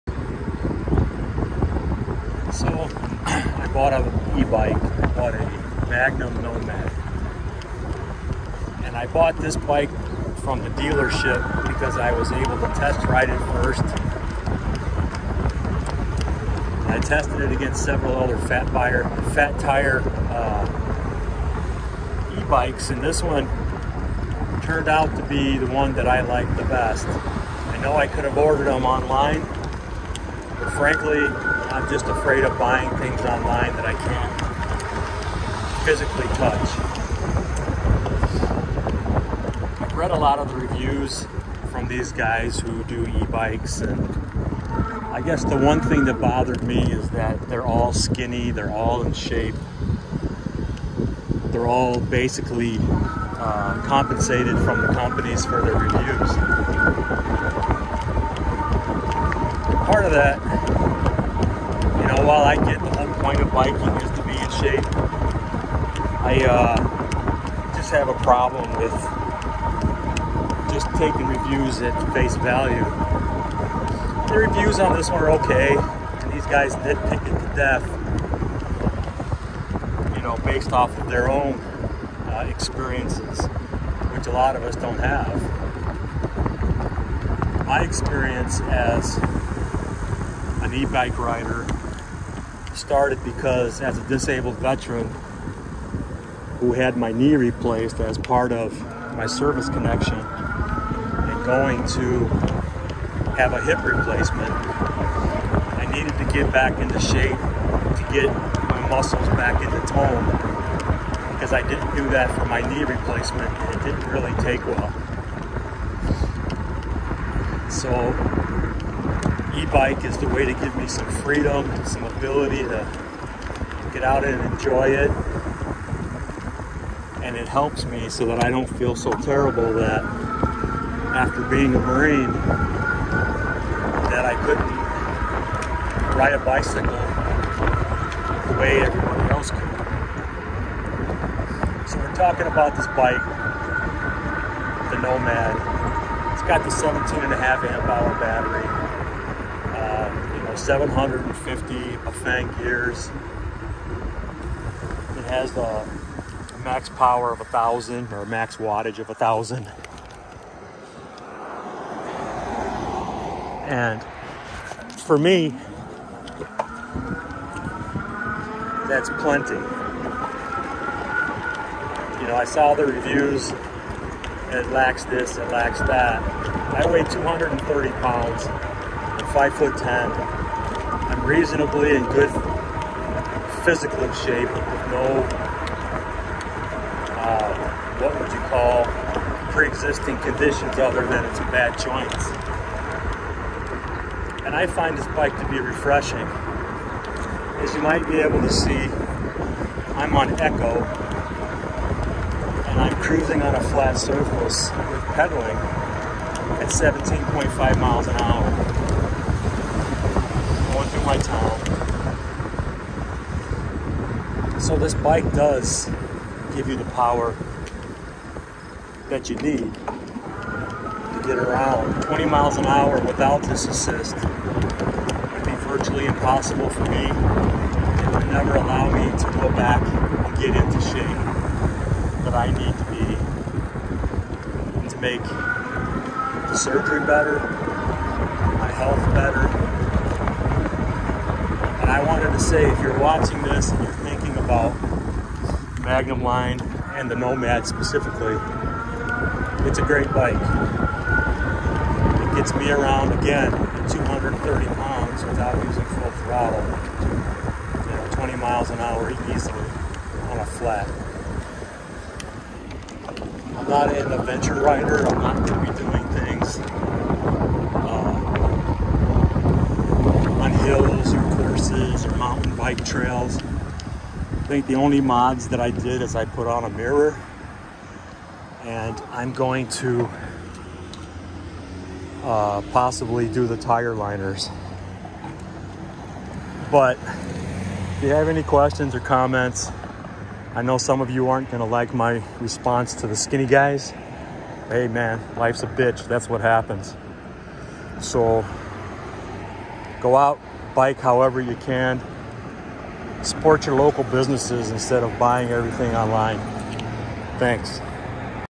New to Ebiking, Started with Magnum Nomad!
While I am a novice, out of shape and by no means an authority, I found that the Magnum Nomad fit my needs for biking and hunting by giving more access to spots I might not otherwise be able to get to quietly.

Not many reviews by guys like me so here mine is for better or worse